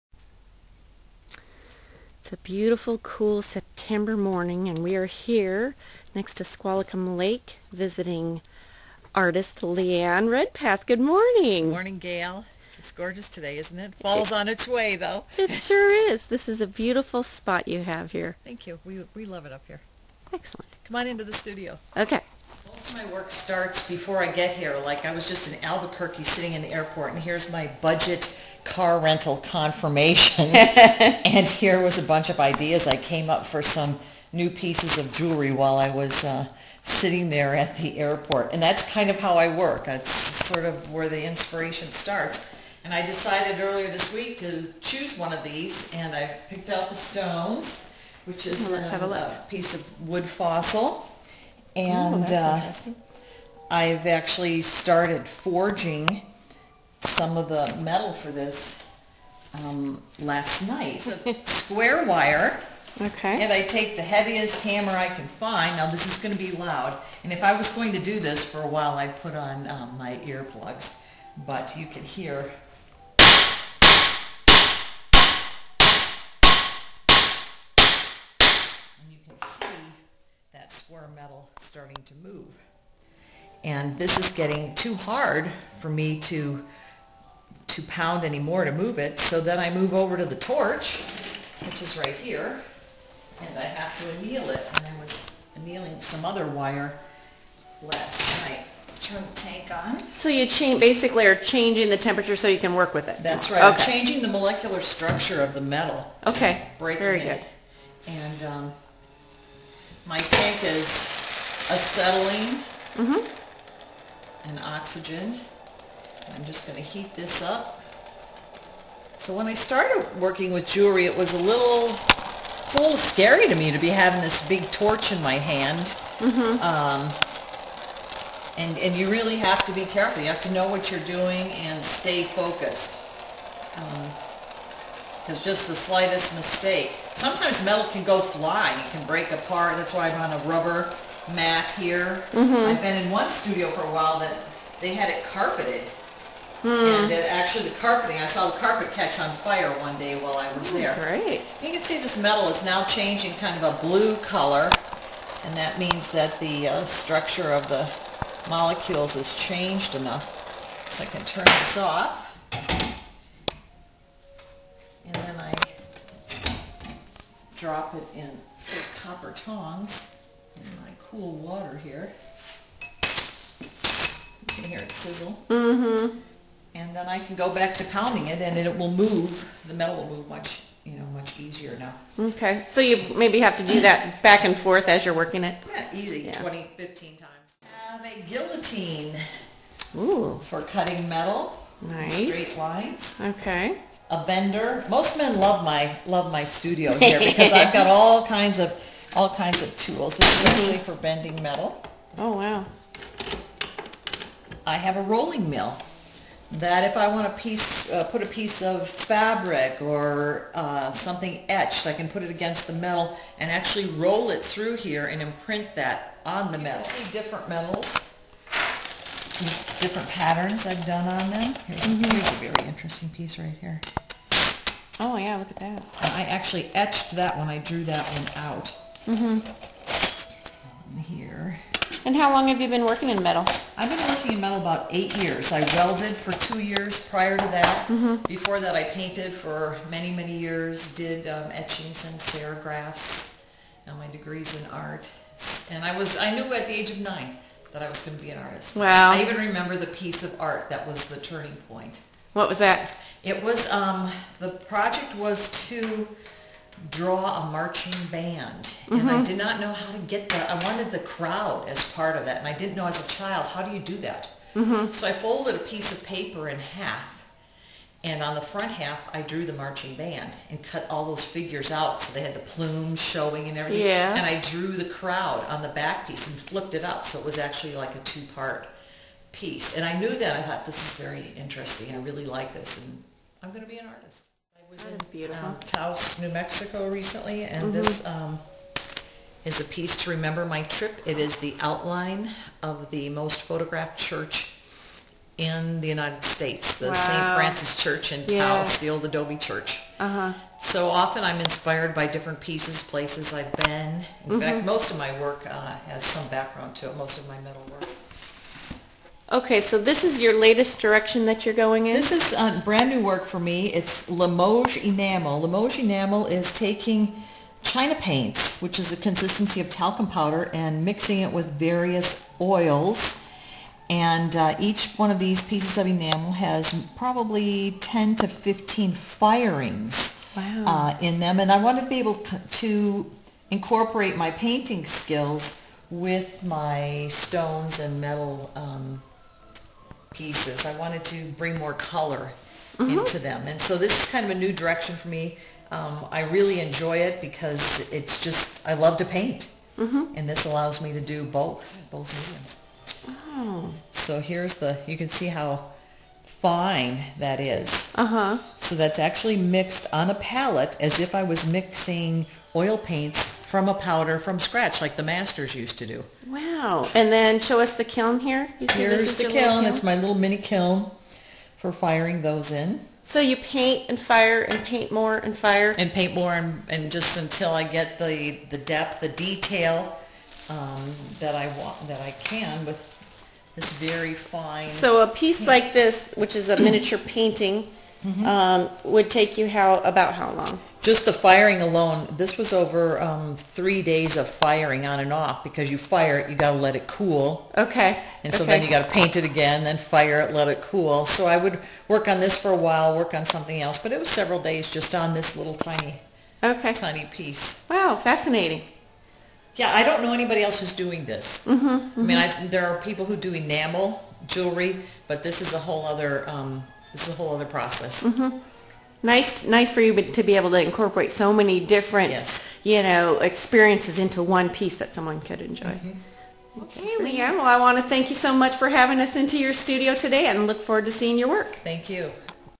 Visit artist Liane Redpath at her Studio
Learn how artist Liane Redpath creates her metal jewelry. Watch how she forges metal, makes patterns in metal, comes up with design inspiration, and uses her latest technique called "Limoges enamel" to make a miniature painting on metal that she incorporates into a pendant. Liane is a metal artist who shows her jewelry at Gallery by the Bay in Stanwood, WA.